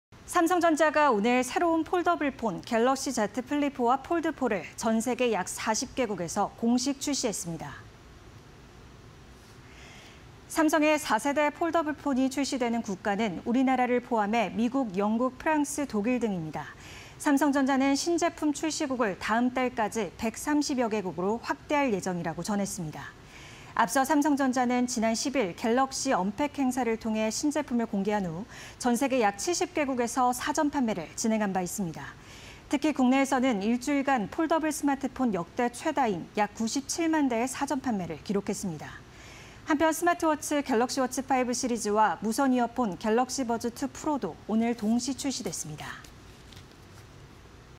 삼성전자, '갤럭시Z 플립4·폴드4' 40개국 공식 출시 (20220826)
삼성전자, '갤럭시Z 플립4·폴드4' 40개국 공식 출시